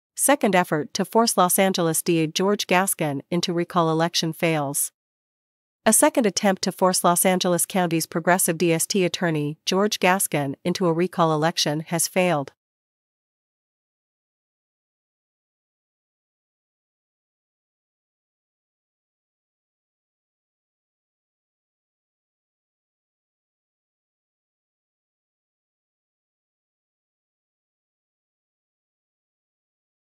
Second effort to force Los Angeles D.A. George Gascón into recall election fails
A second attempt to force Los Angeles County's progressive Dist. Atty. George Gascón into a recall election has failed.